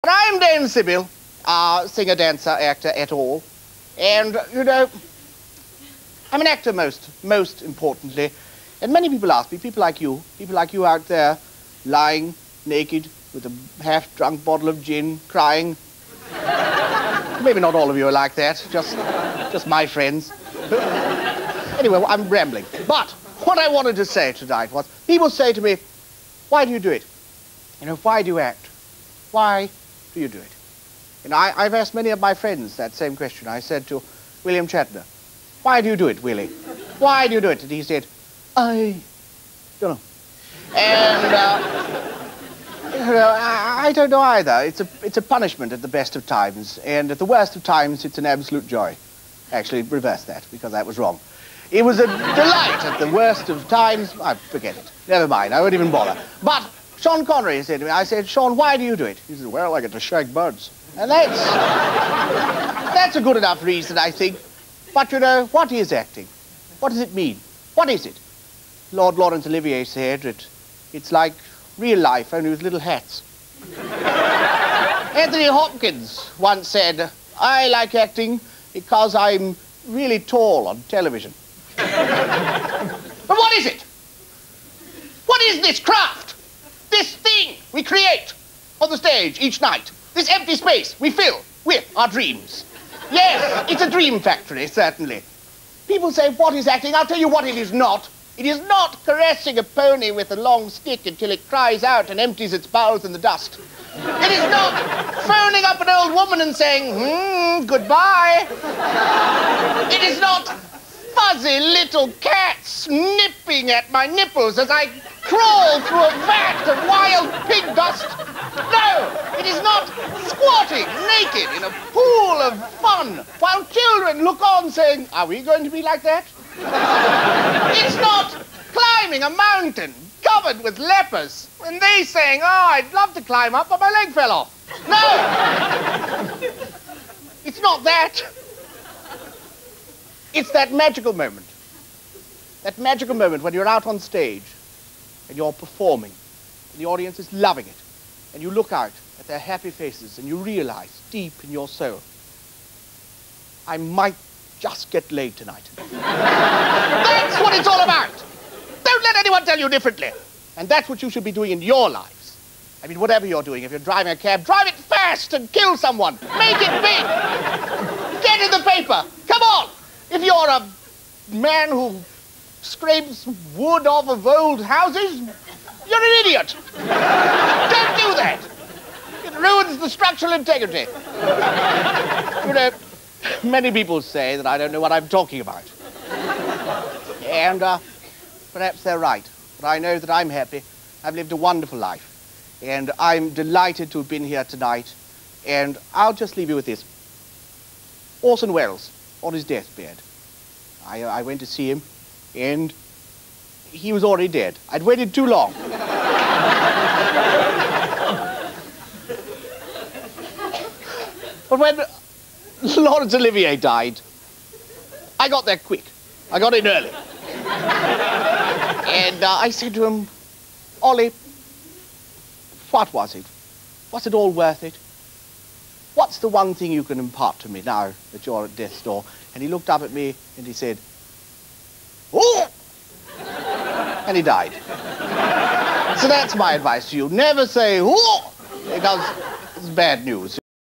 Sean Cullen as "Dame Sybil"
The great Canadian improv artist, in '98.